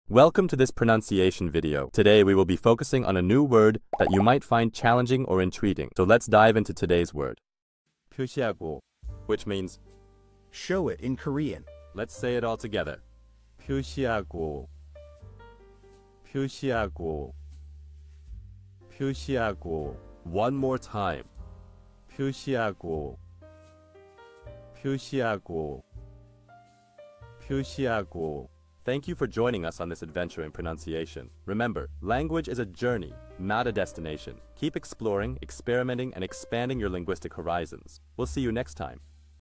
How to pronounce pyosihago | 표시하고 (Show it in Korean)
Master the Pronunciation of 'pyosihago | 표시하고' - which means : Show it in Korean 📝 with @PronunciationKorean 🎓🗣️ - [Your Guide to Korean]

Learn to pronounce 'pyosihago' perfectly in Korean with our easy and engaging tutorial by @PronunciationKorean! 📖📽️ In this comprehensive video, we provide you with native speaker examples 🗣️👂 and phonetic breakdowns 🎵 to ensure you get the pronunciation just right.